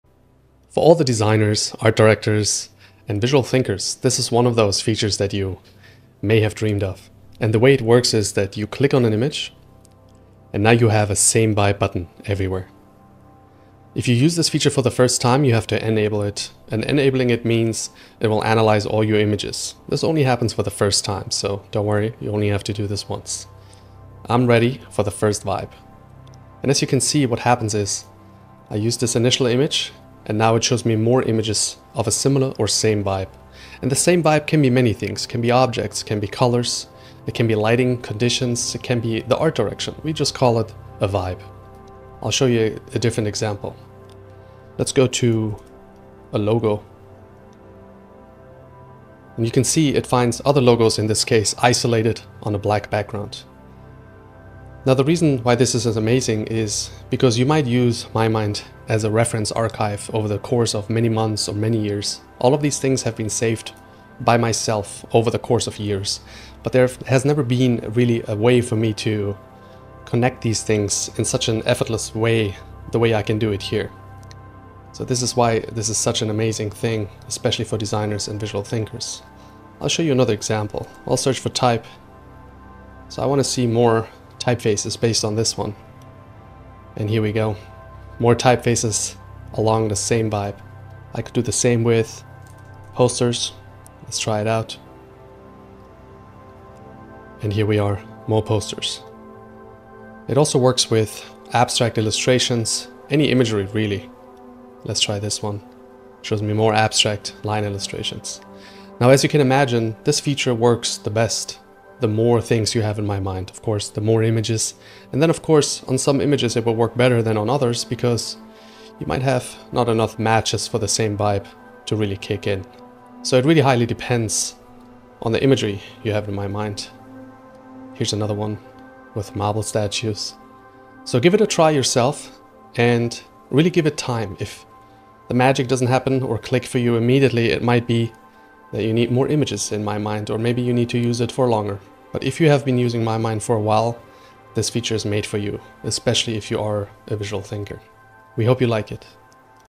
Reveal connections in your mind with Same Vibe
The new Same Vibe feature analyzes images you've saved and collects others from your mind with a similar look and feel.

It's impressively precise and even recognizes the tone or sentiment of your images.

The more images in your mind, the better it works!

Sign up for our Mastermind plan to try it: mymind.com